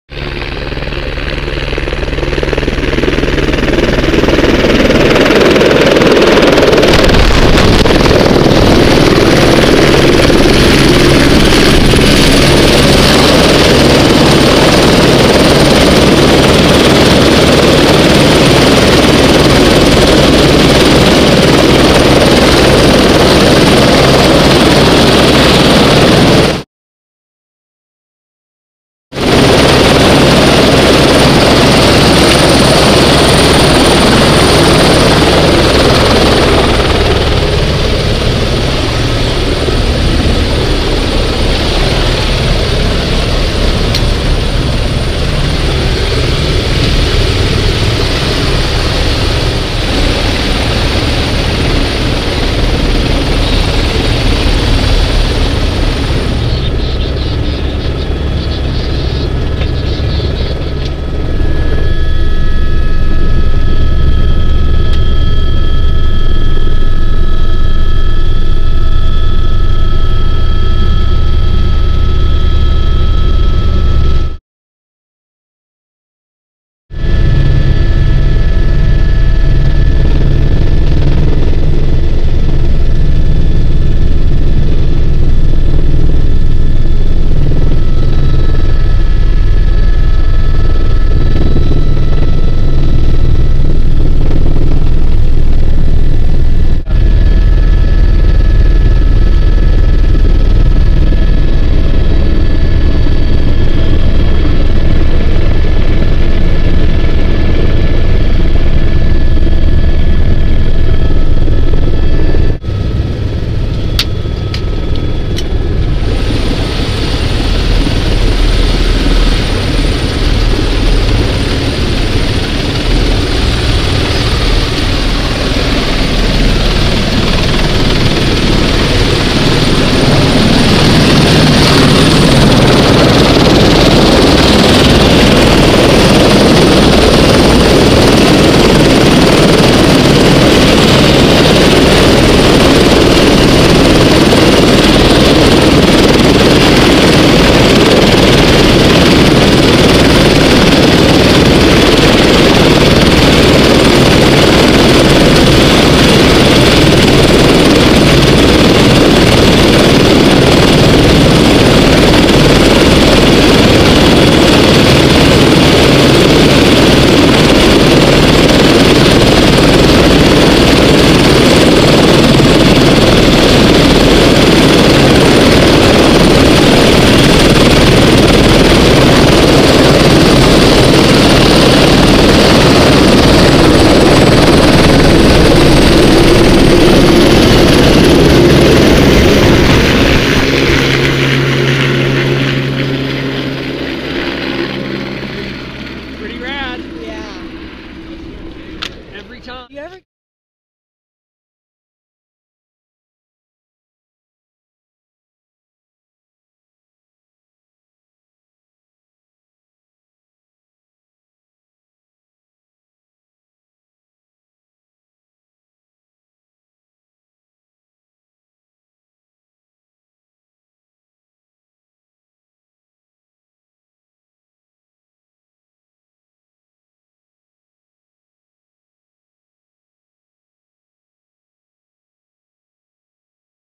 Heli ride in AK
Even when the snow is less than perfect, riding a heli in AK is one of the most amazing experiences in skiing.